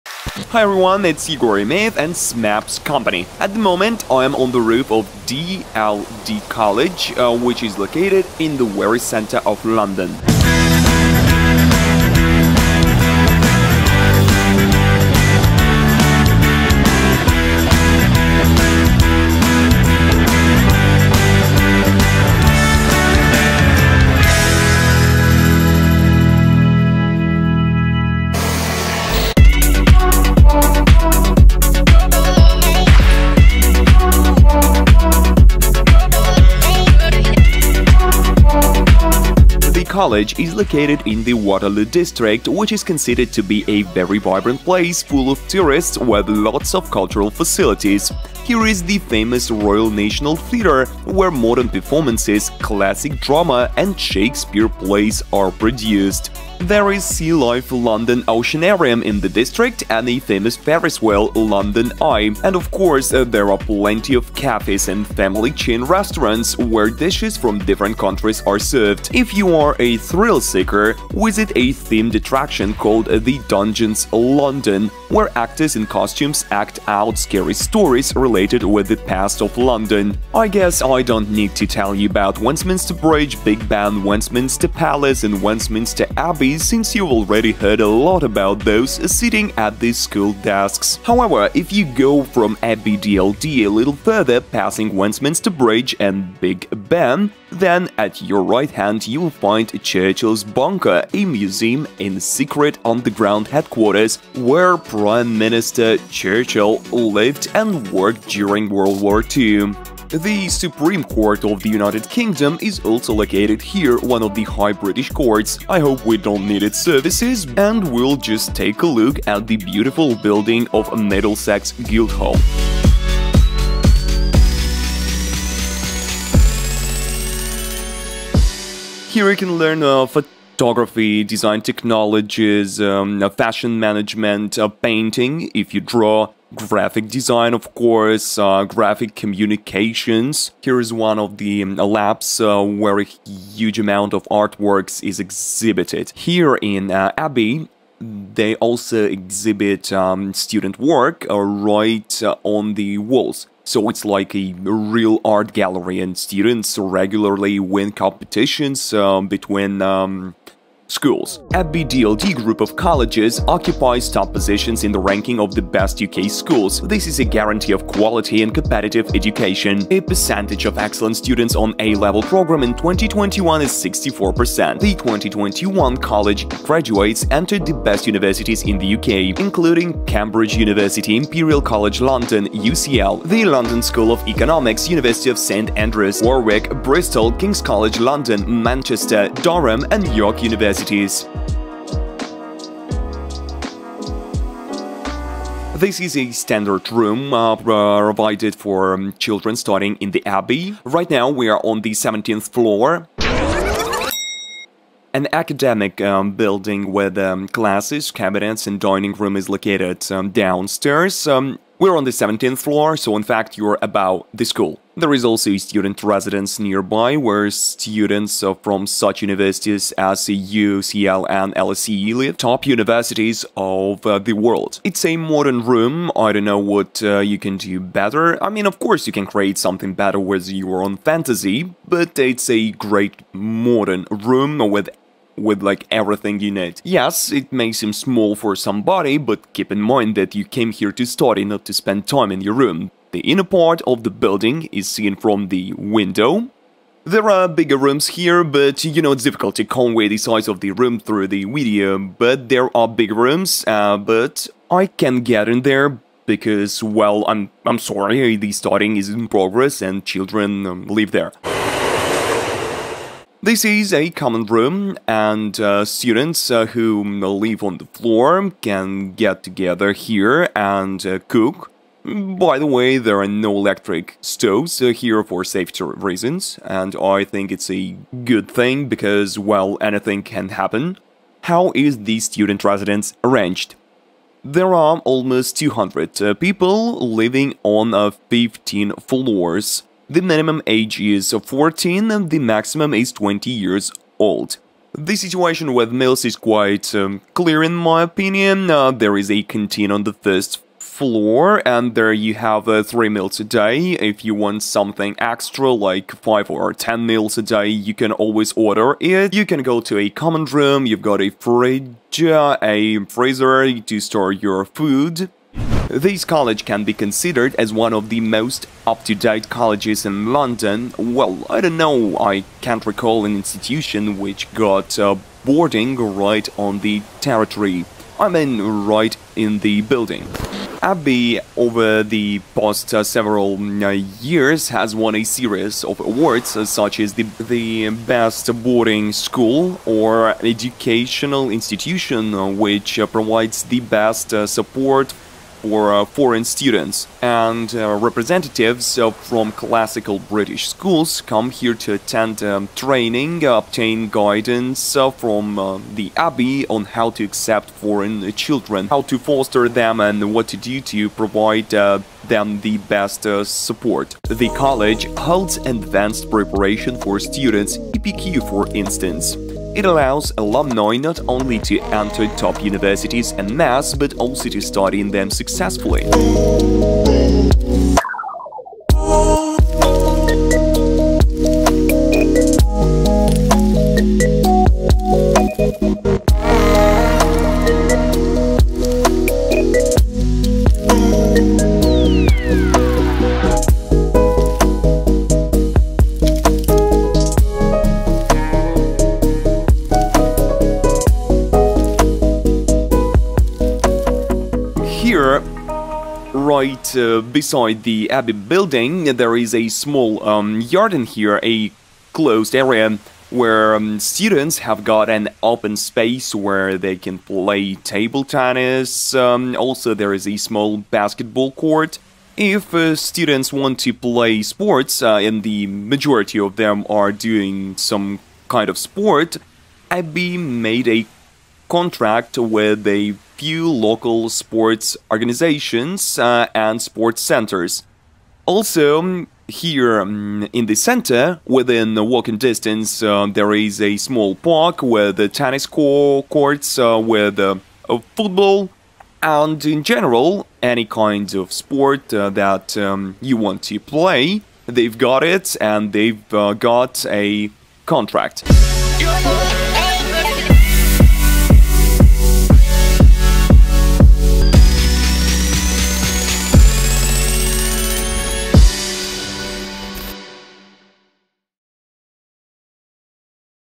The best education in England. Abbey DLD London school: apply to Abbey London College
School in central London: 64% excellent students and contracts with sports organizations. More about all this in the video!

Welcome to the Smapse Education channel! A college in central London, Abbey DLD is a college with 64% excellent students. Students ages 14 to 20 come here to get a high education in England. Abbey College provides dormitory accommodation right in the school building.

Graduates of DLD College London enter the TOP world's universities. Elite college in England allows engaging in any sport. For this purpose, the college has a contract with several sports organizations. Find out more about this and other advantages of Abbey DLD College London in the video.

In the video:
0:00 Abbey College London.
0:49 Waterloo District.
2:10 What is being studie at Abbey DLD College London private school?
3:16 Dormitory at a private school in London.
15:17 Most modern college with awards for best education.
6:35 About Abbey College sports.